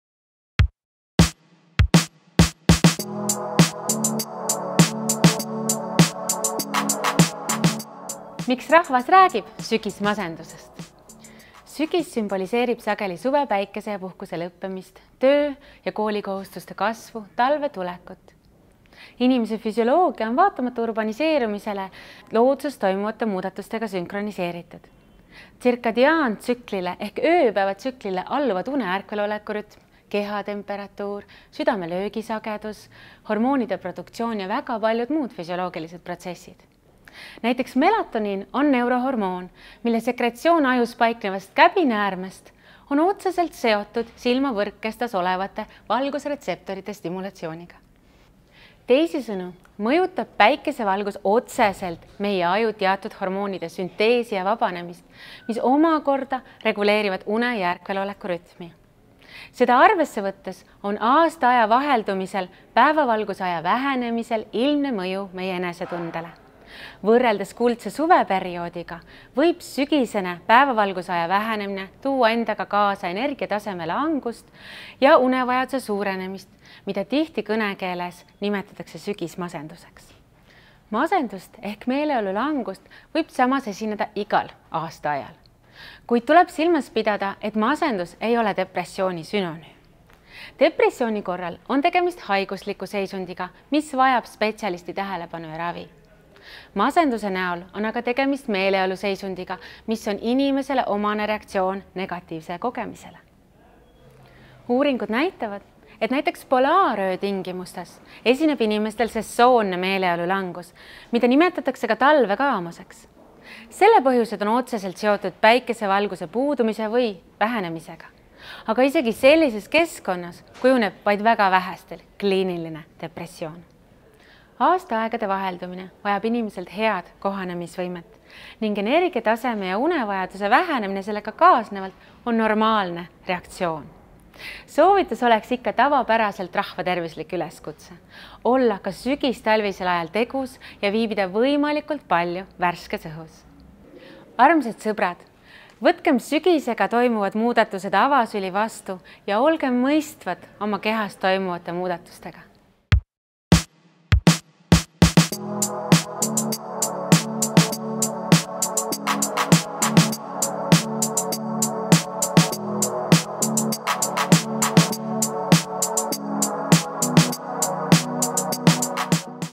Teadlane teab: Mis on sügismasendus? (Merit Kudeviita)
Sügis sümboliseerib sageli suvepäikese ja puhkuse lõppemist, töö ja koolikohustuste kasvu, talve tulekut ja nii edasi. Kuidas see kõik meid endid mõjutab, räägib TLÜ tervisekäitumise ja heaolu doktorant Merit Kudeviita.